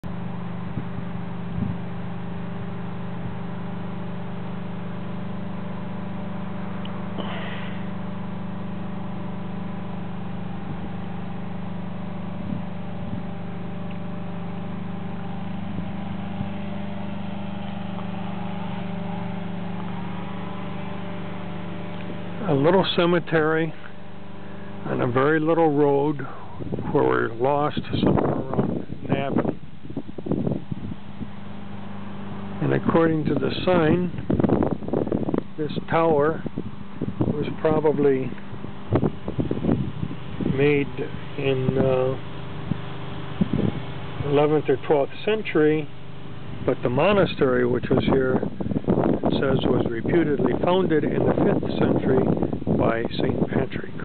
The lost graveyard
After leaving Newgrange we headed for Trim. However, due to a shortcut we became lost (this happened regularly; we sort of liked it.) We happened upon this graveyard.